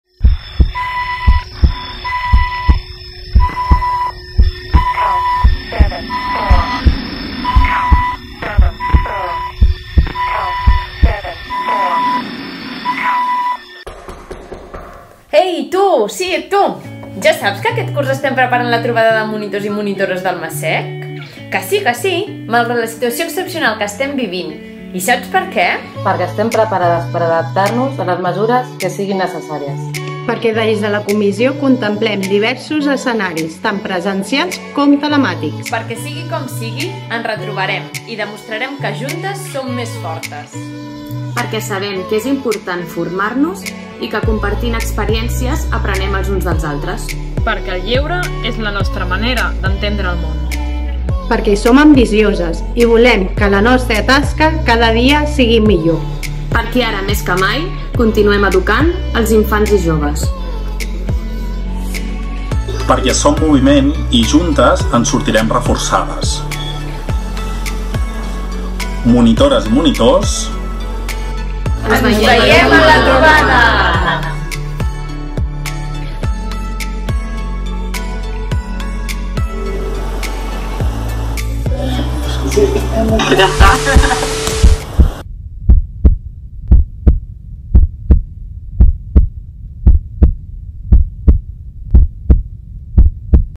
Ens retrobarem!: escalfem motors per la Trobada de Monitors/es 2021
La comissió organitzadora de la Trobada de Monitors/es 2021 està treballant en diversos escenaris perquè, els propers 13 i 14 de març, tots i totes puguem gaudir (malgrat la covid-19, de manera segura) d'un dels esdeveniments més esperats de l'any pels membres del Moviment!